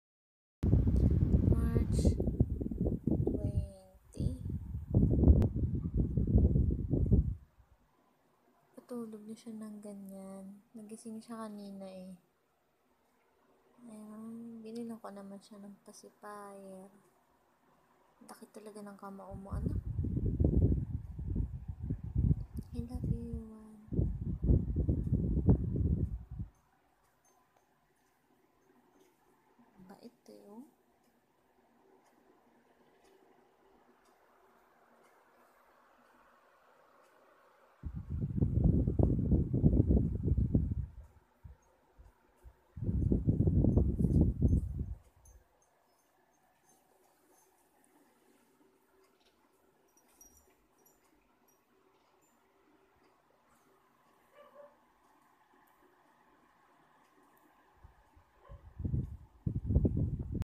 Thumbs suck
Nagising sya eh! Pag nagigising sya yung kamay nya palagi nauunang hinahanap nya.. Sabi nila.. Mabait daw pag nagtathumbsuck ang baby